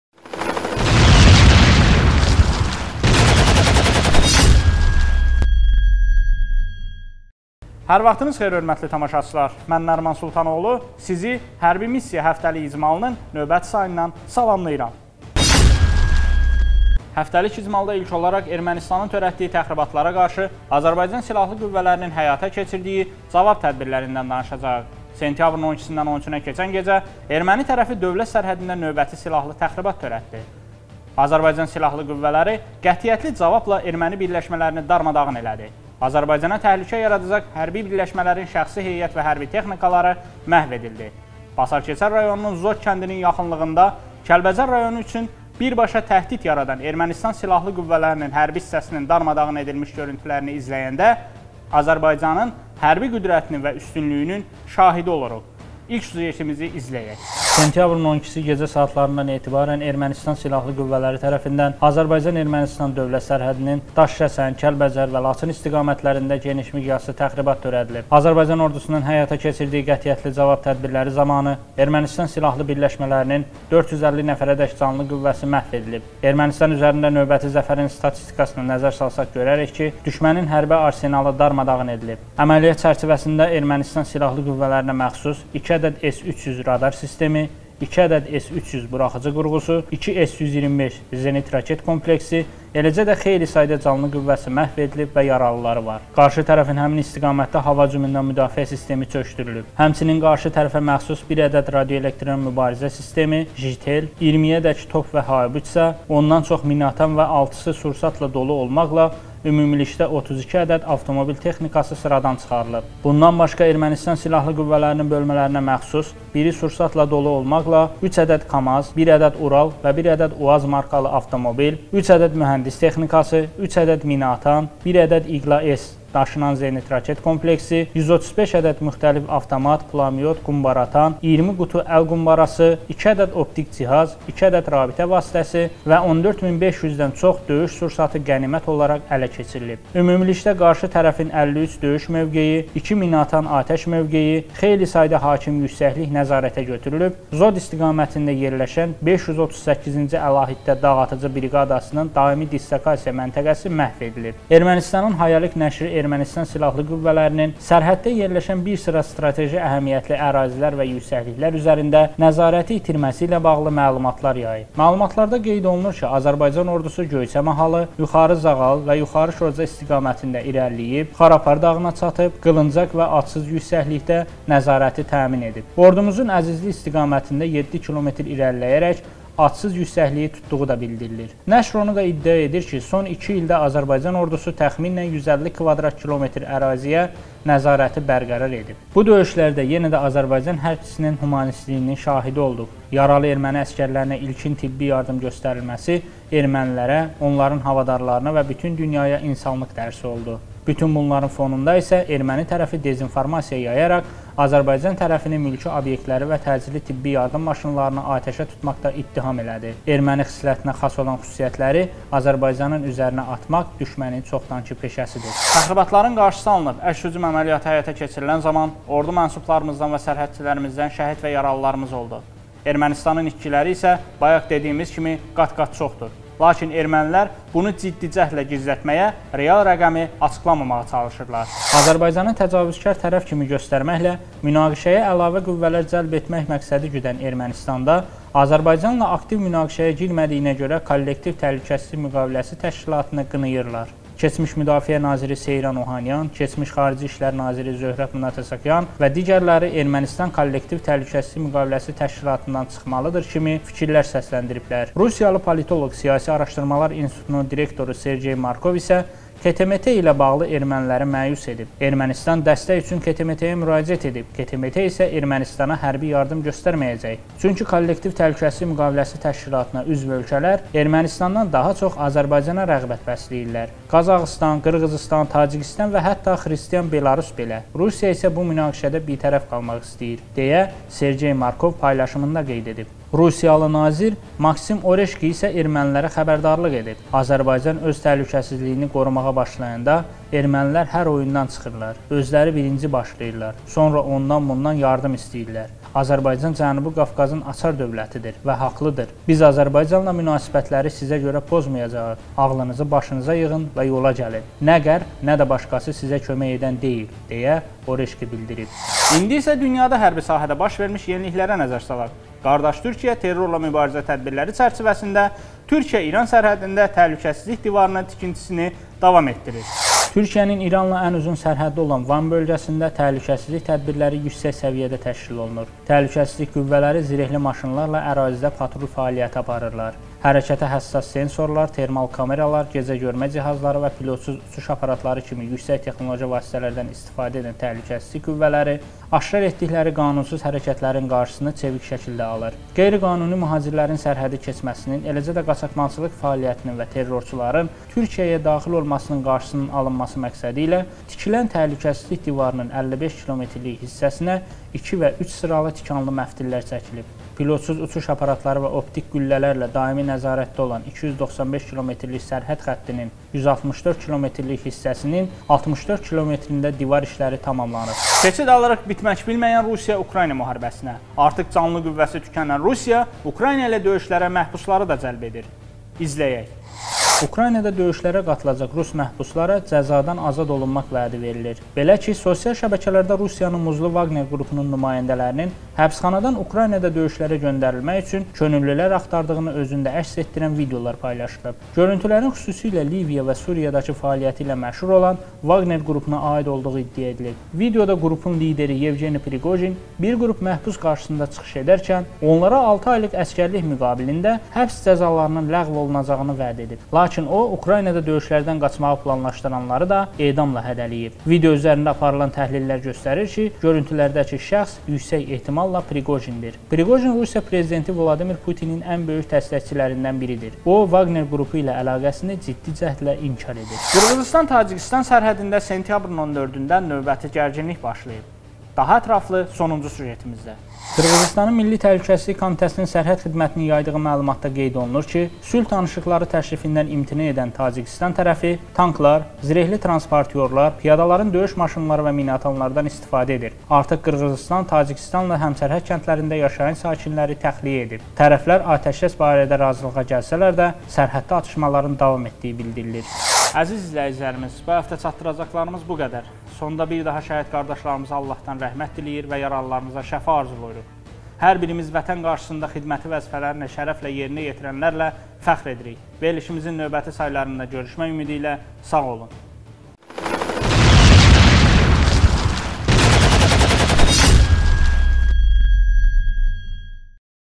🔴 HƏRBİ MİSSİYA - HƏFTƏLİK İCMAL
✅ Ermənistanın hərbi birləşmələri məhv edildi
✅ KTMT Ermənistana hərbi yardım etməyəcək
✅ Türkiyə sərhədləri etibarlı mühafizə olunur
✅ Rusiya Ukraynada döyüşlərə məhbusları cəlb edir
✅ Qırğızıstan-Tacikistan sərhədində növbəti qarşıdurma